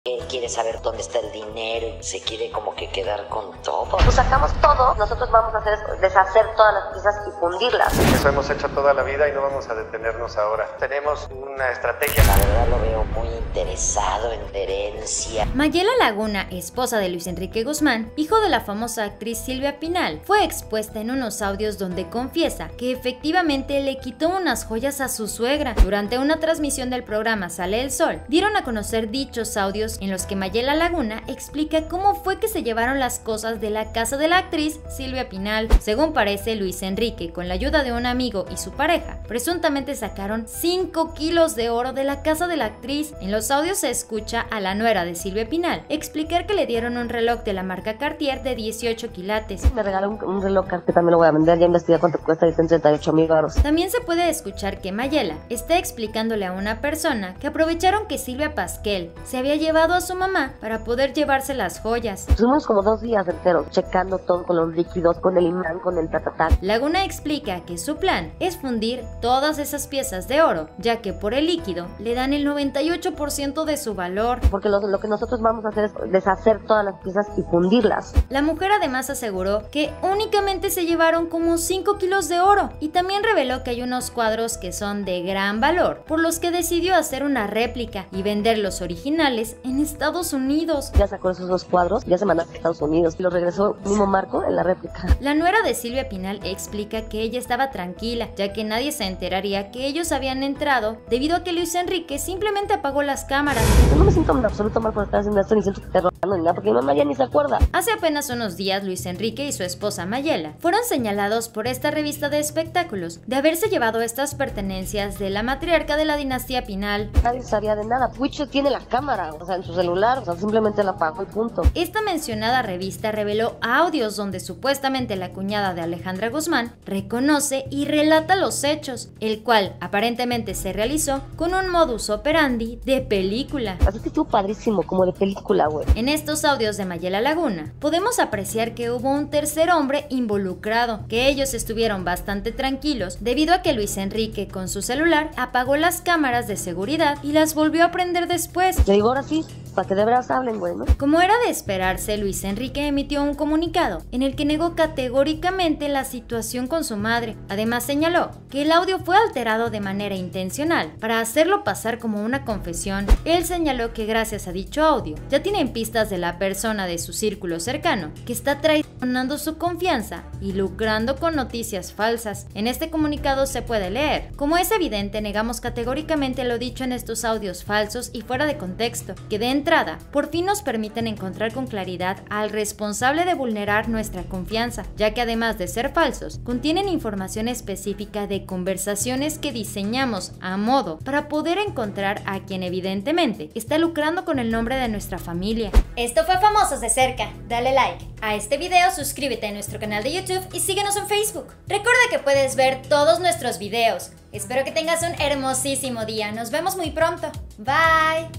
🔥🔥 𝗡𝗼 𝗘𝘀𝗽𝗲𝗿𝗮𝗿𝗼𝗻 𝗾𝘂𝗲 𝗙𝗮𝗹𝗹𝗲𝗰𝗶𝗲𝗿𝗮 𝗟𝘂𝗶𝘀 𝗘𝗻𝗿𝗶𝗾𝘂𝗲 𝘆 𝗦𝘂 𝗘𝘀𝗽𝗼𝘀𝗮 𝗦𝗮𝗾𝘂3𝗮𝗿𝗼𝗻 𝗧𝗼𝗱𝗼 𝗱𝗲 𝗟𝗮 𝗖𝗮𝘀𝗮 𝗱𝗲 𝗦𝗶𝗹𝘃𝗶𝗮 𝗣𝗶𝗻𝗮𝗹 😱
Salen audios de Mayela Laguna narrando como se llevaron todas las joyas de valor de Silvia Pinal de la forma que entraron a la casa para llevarse el oro y luego fundirlo, tambien explica que se llevaron muchos cuadros valiosos de doña Silvia Pinal.

Bitcoin:
Trading:
ecommerce: